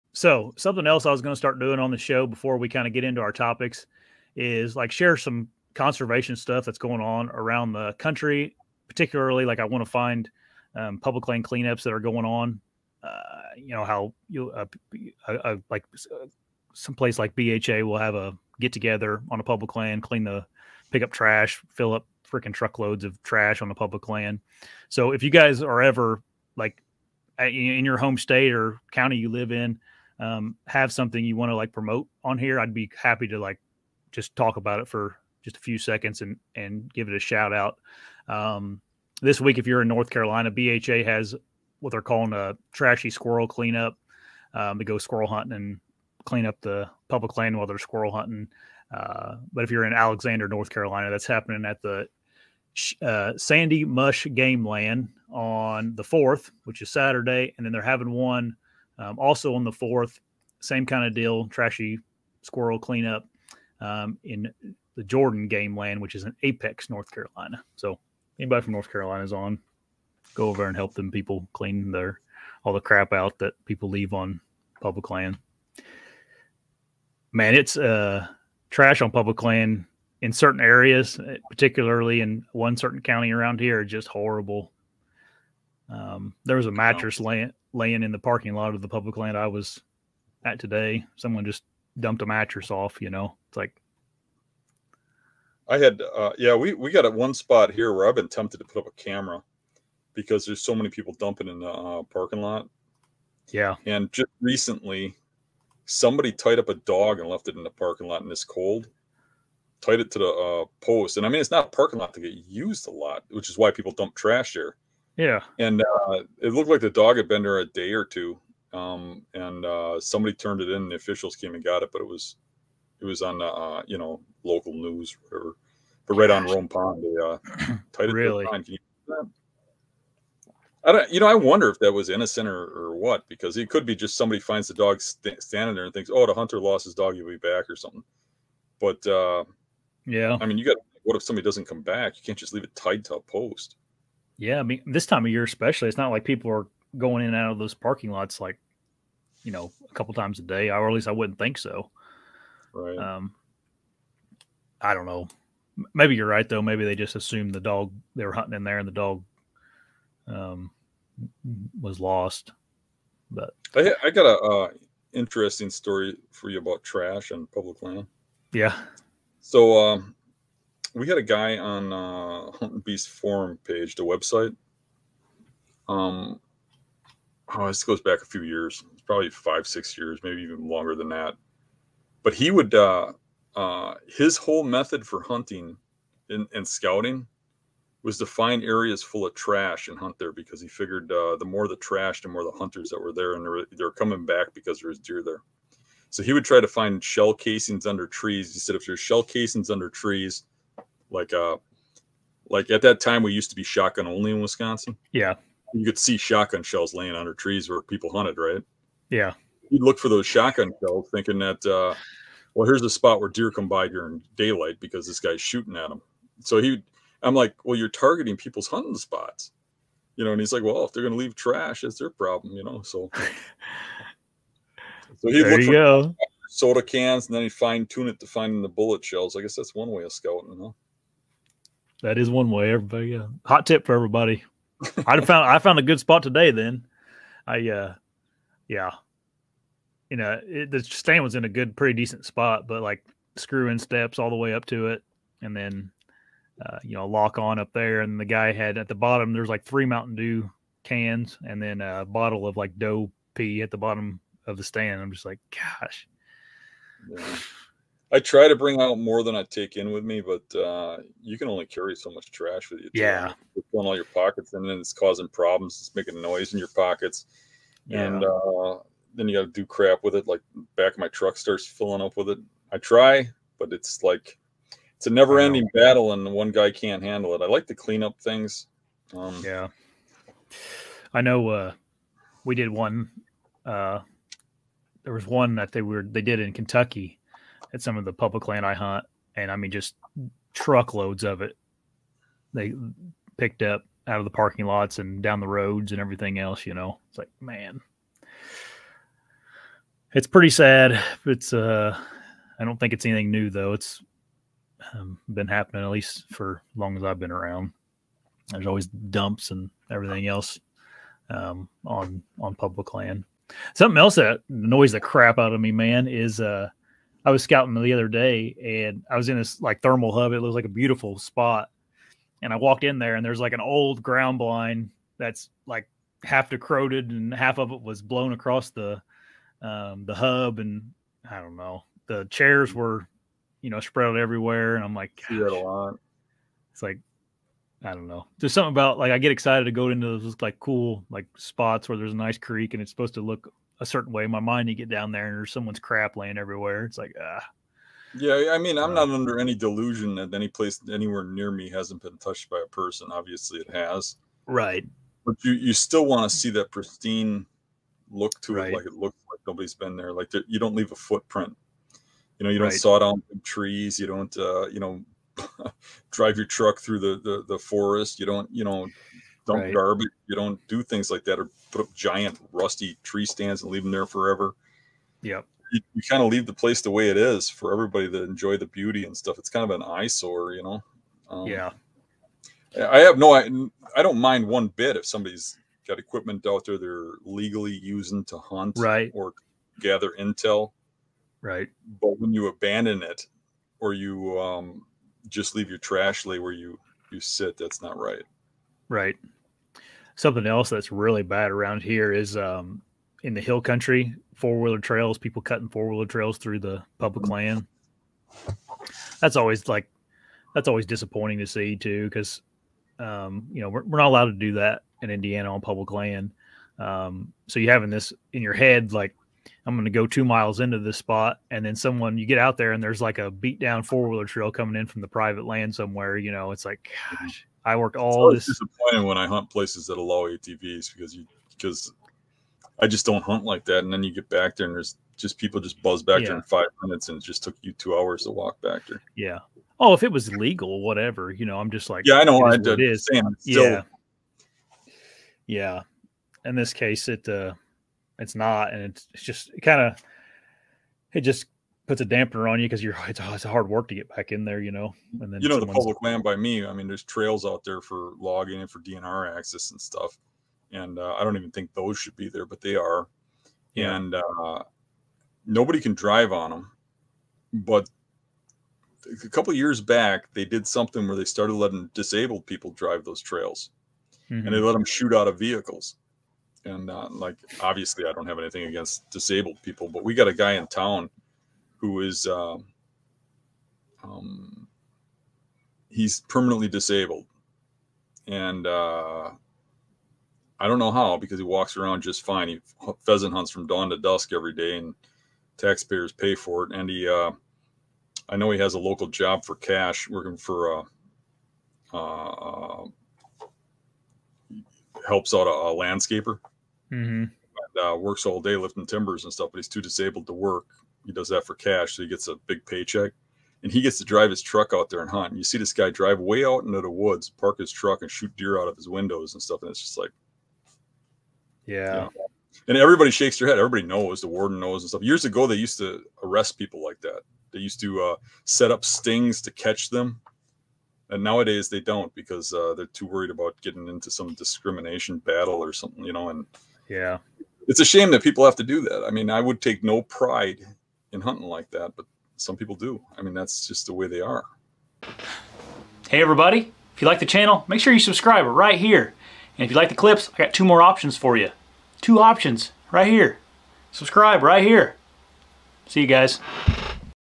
How much trash is on your public lands?
This is a clip from Episode #51 Dan Infalt and Josh Teulker discuss public land cleanups and the issue of trash on public lands.

If you want to see our hunts head over to The Hunting Beast YouTube Channel here